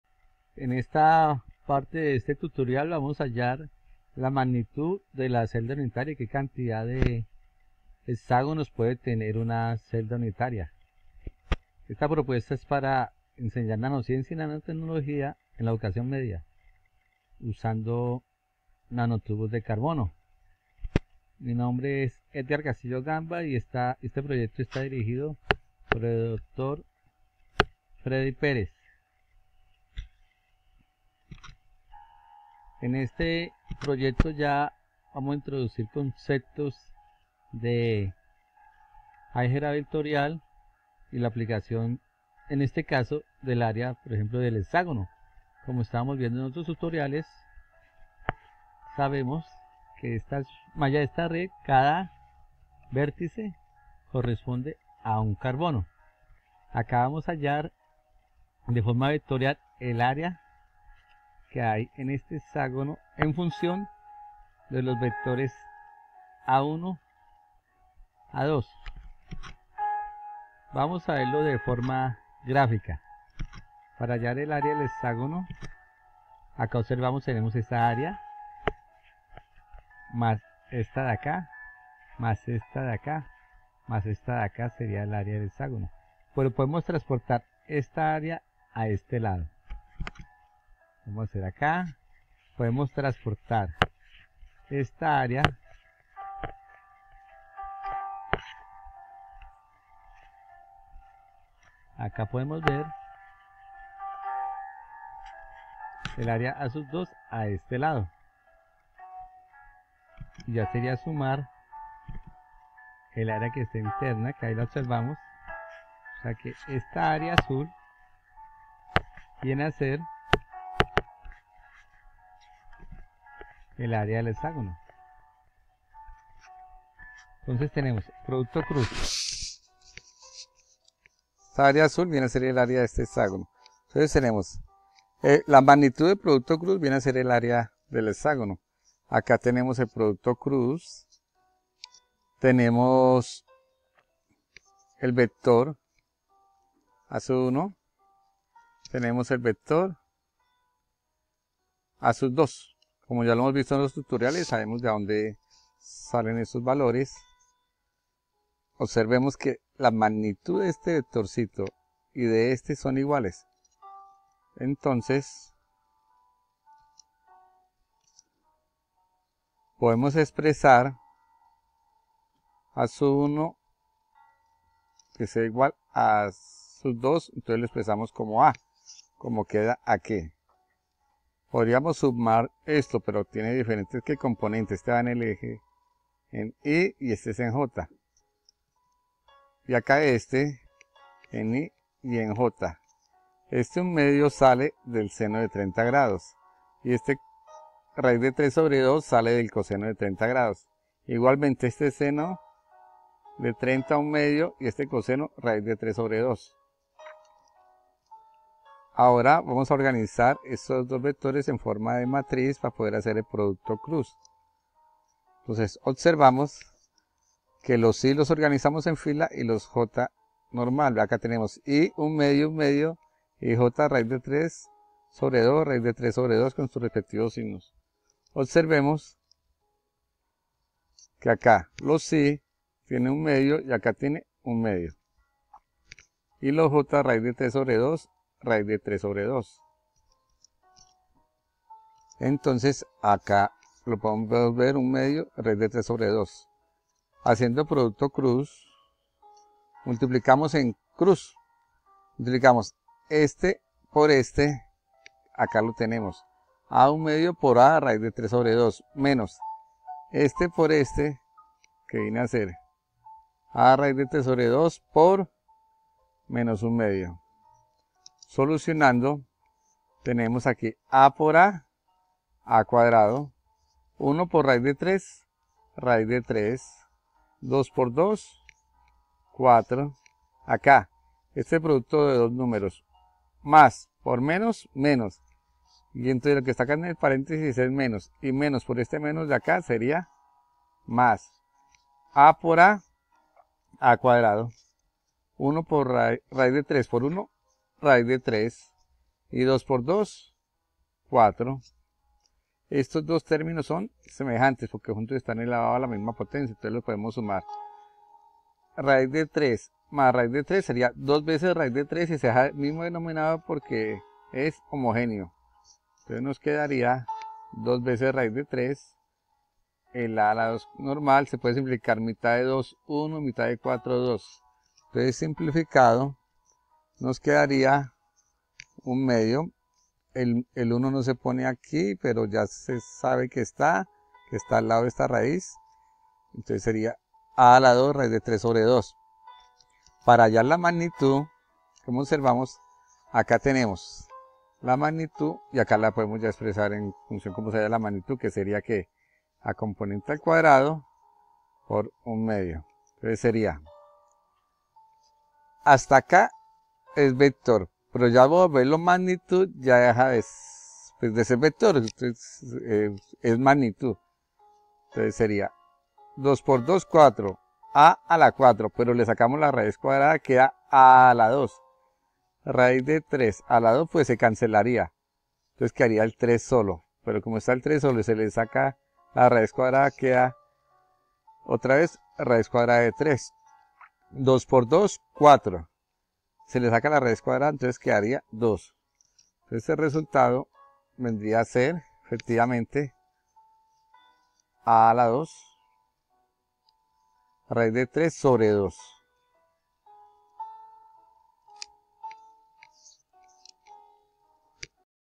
ÁREA DE CELDA PANAL DE GRAFENO
En este vídeo se demuestra el área de la celda panal, desde una perspectiva geométrica, y utilizando producto cruz, magnitud de un vector y reglas algebraicas.